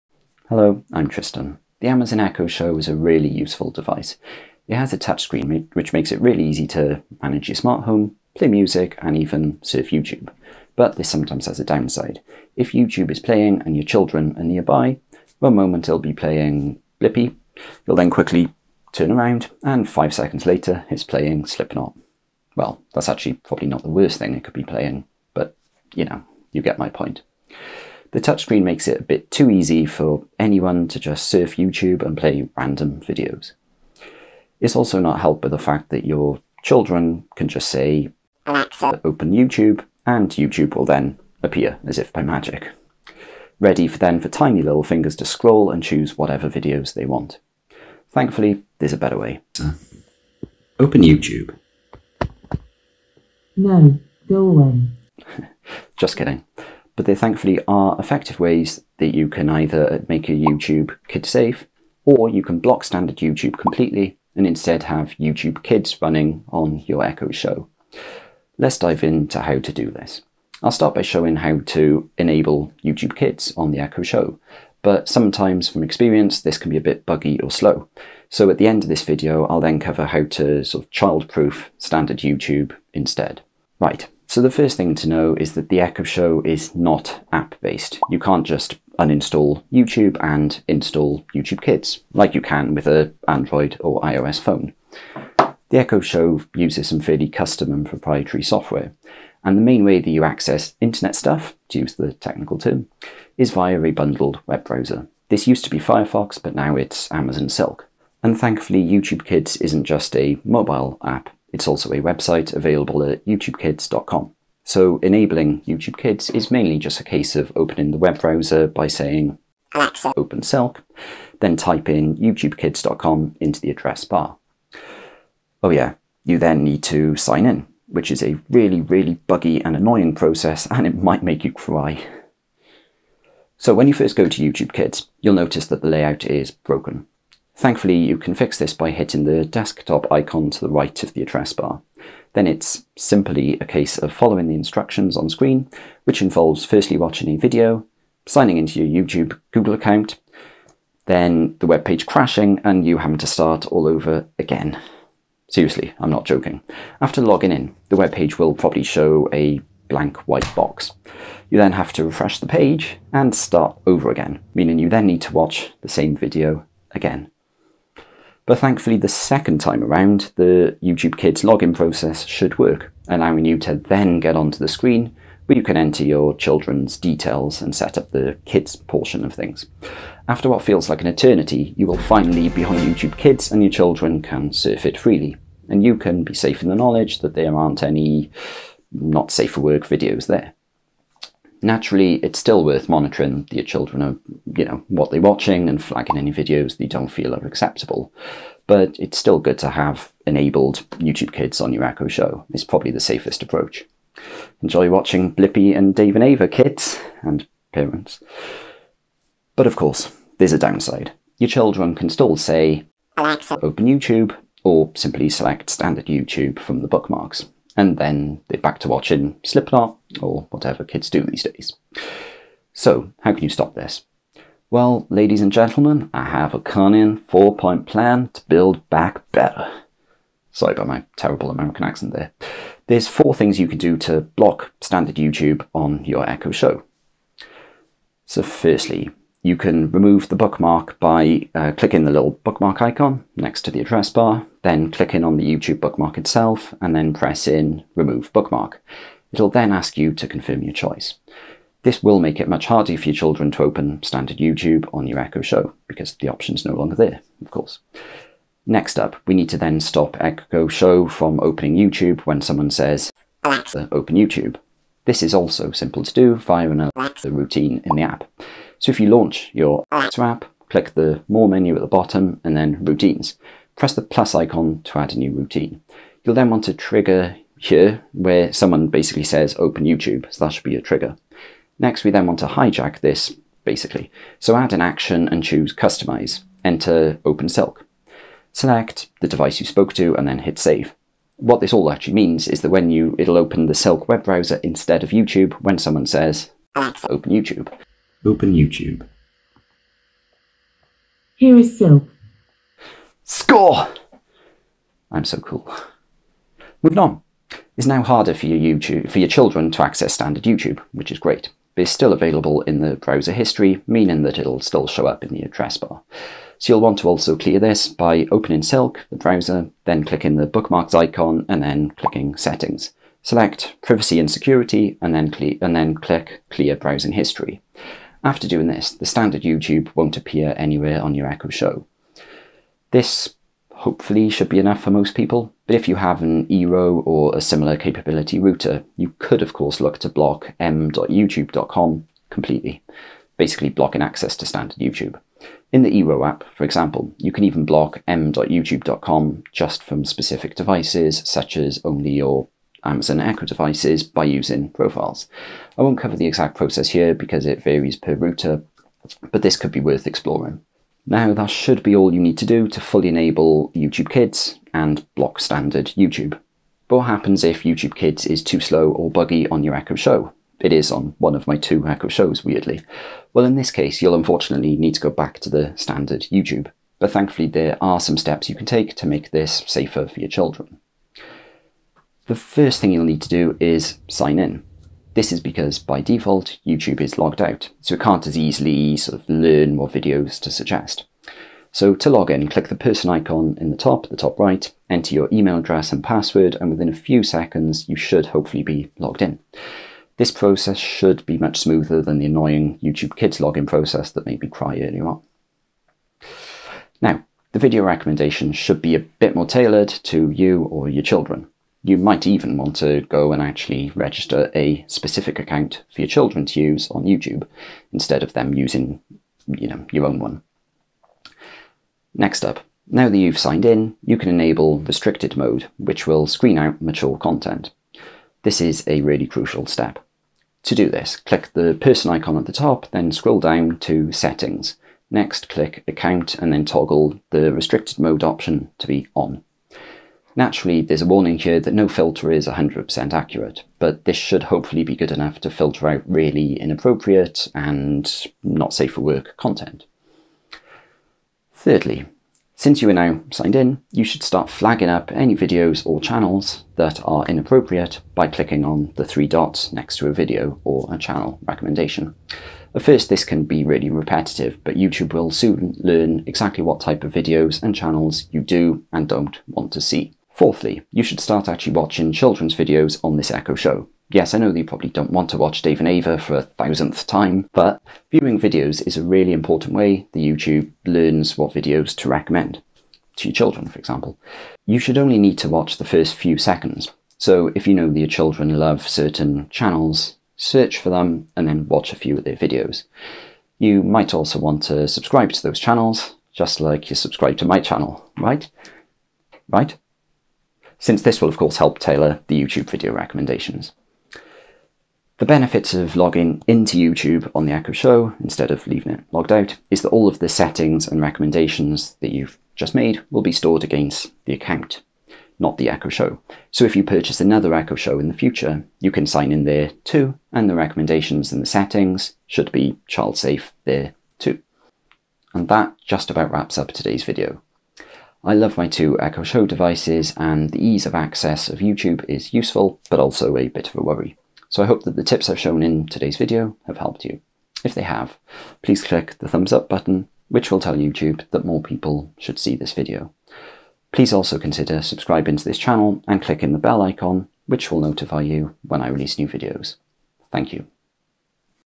Make YouTube Kid-Safe On The Amazon Echo Show (A Simple Tutorial Guide)
The Echo Show is a great touchscreen-enabled product from Amazon, but it also makes it a little bit too easy for children to freely surf YouTube - potentially accessing inappropriate videos.

Therefore I went looking for makes on how to child-proof the YouTube installed on the Echo Show, and this video covers everything you need to know. It covers how to make standard YouTube "child safe", along with how to "enable" YouTube Kids on your Echo Show

Both options can work well, but can also have some flaws too - which I discuss in this video.

0:00 Intro
1:12 How to 'enable' YouTube Kids
2:06 Fixing the annoying registration process
3:00 Benefits of YouTube Kids
4:02 Remove standard YouTube bookmark
4:18 Block open YouTube voice command
5:05 Clear browsing history
5:36 Block m.youtube.com in your router
6:04 Make standard YouTube child-safe
6:21 How to sign-in to YouTube
6:47 Video recommendations
7:04 Restricted Mode
7:36 Mark inappropriate
7:54 Watch some children's videos
8:24 Benefits of logging into YouTube
8:45 Wrapping Up